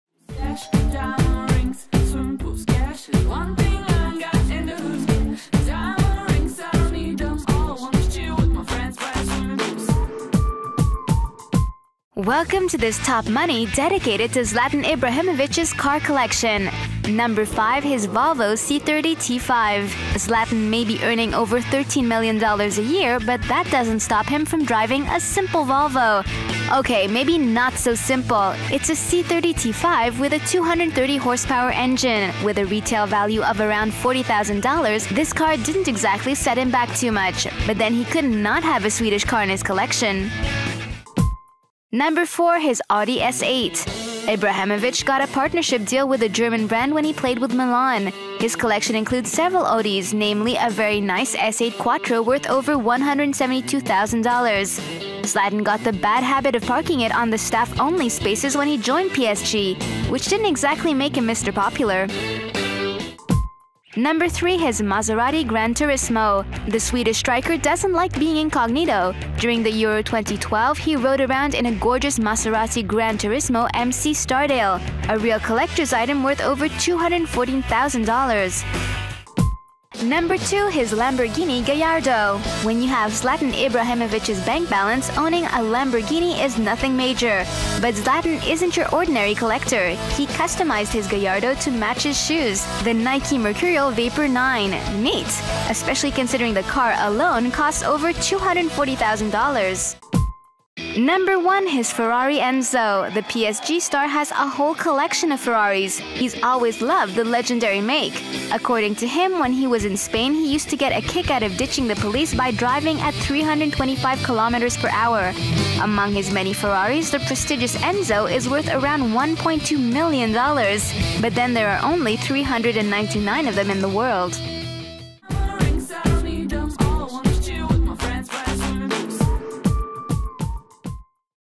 Zlatan Ibrahimovic Car Collection
Check out what's inside Zlatan Ibrahimovic's garage:

5. Volvo C30 T5
4. Audi S8
3. Maserati Gran Turismo
2. Lamborghini Gallardo
1. Ferrari Enzo